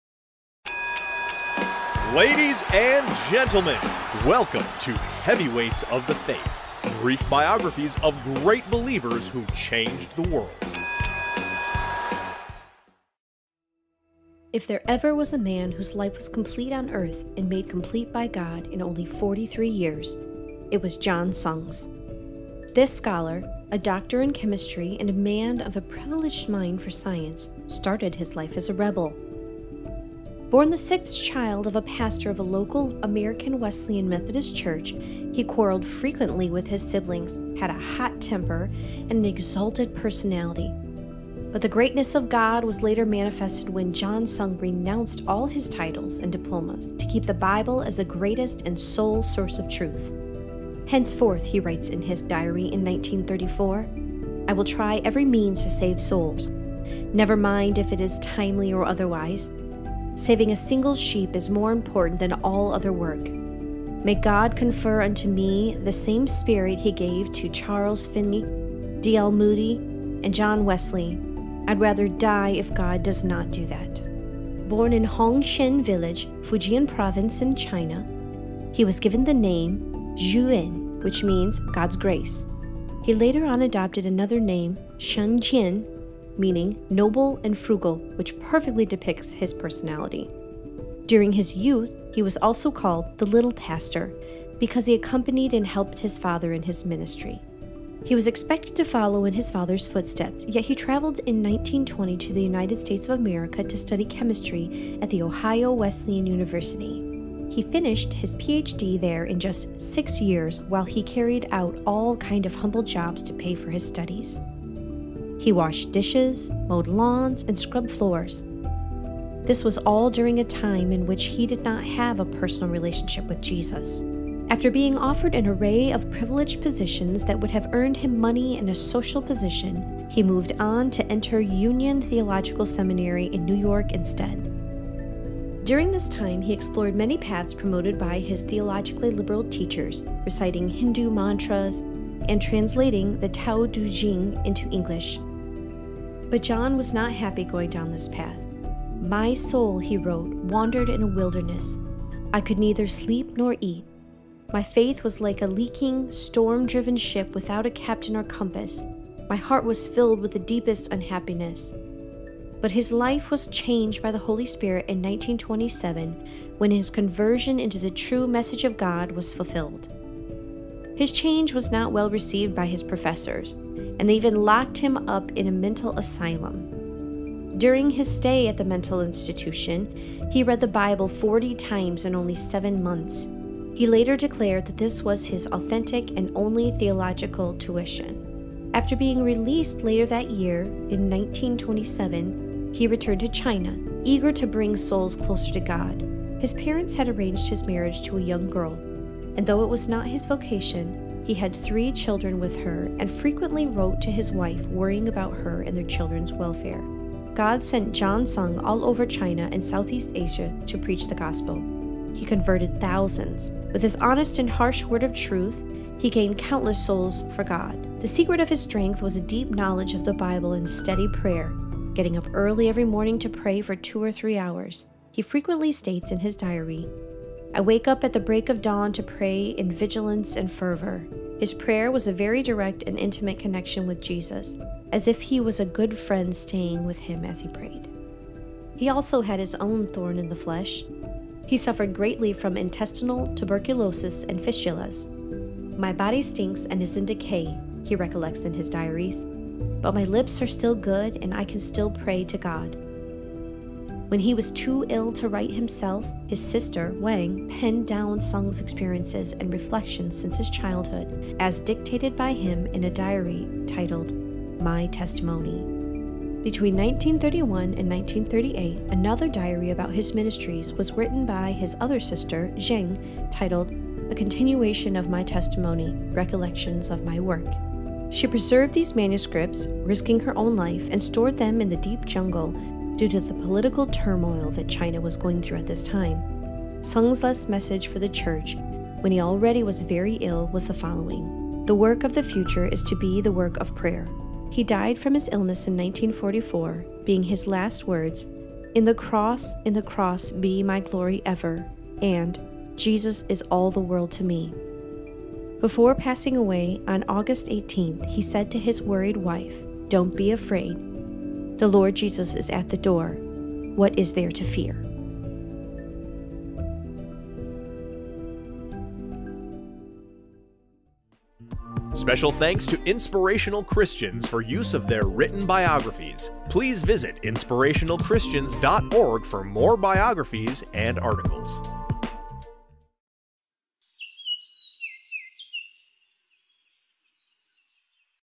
John Sung | Heavyweights of the Faith
John Sung boldly proclaimed Jesus in China and won thousands to Christ. Heavyweights of the Faith! Brief biographies of great believers who changed the world.
Music